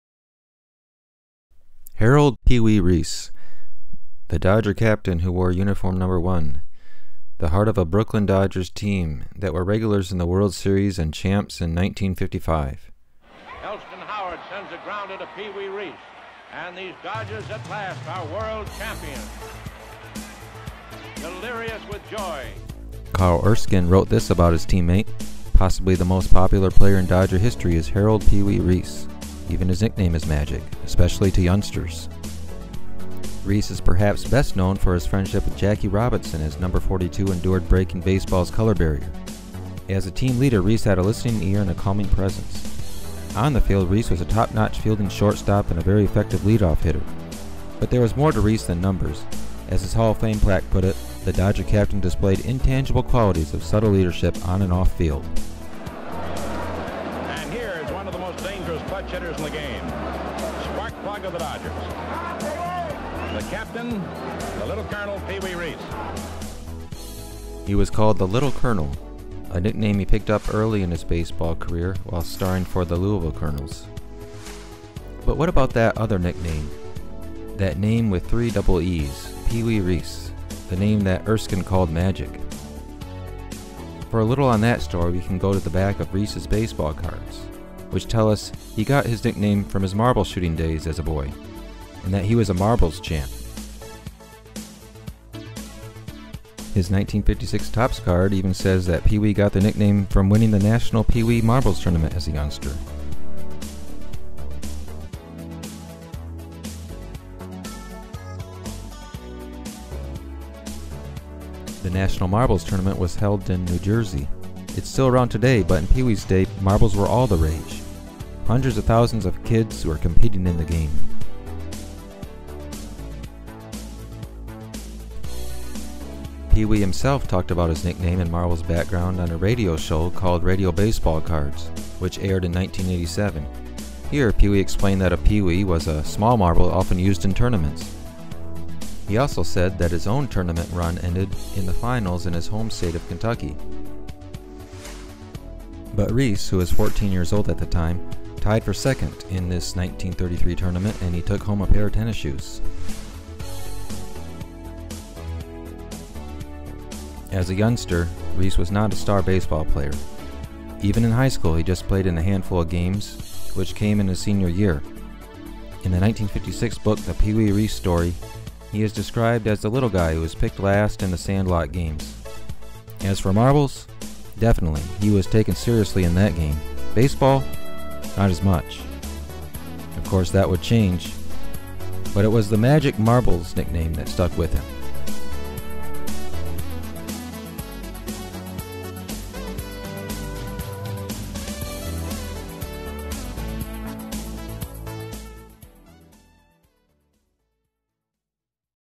How "Pee Wee" Reese Got His Name
Pee Wee Reese: Baseball star and marbles champ!

Special thanks to Luke ("the Captain") for putting this video together.